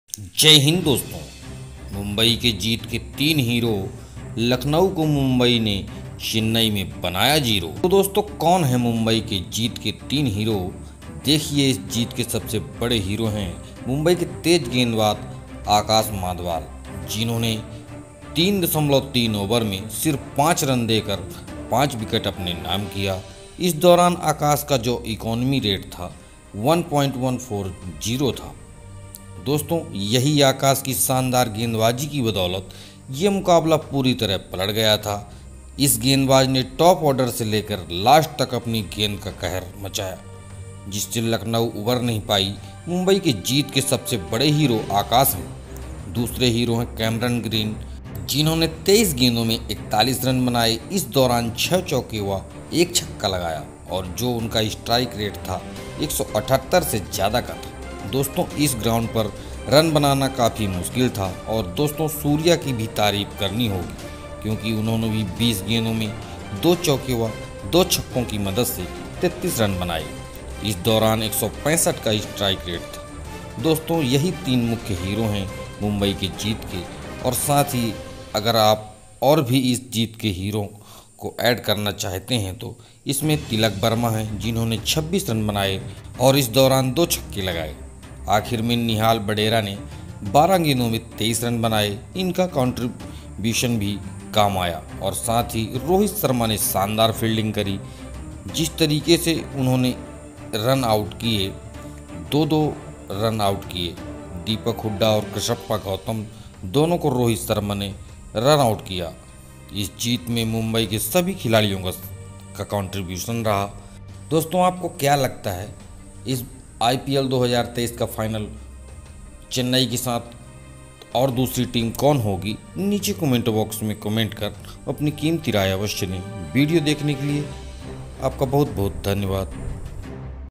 IPL 2023 :- मुंबई के जीत के 3 Hero, लखनऊ को बनाया Zero |Qualifier|CSK|Akash Madhwal|Suryakumar|Rohit
Gulab Cricket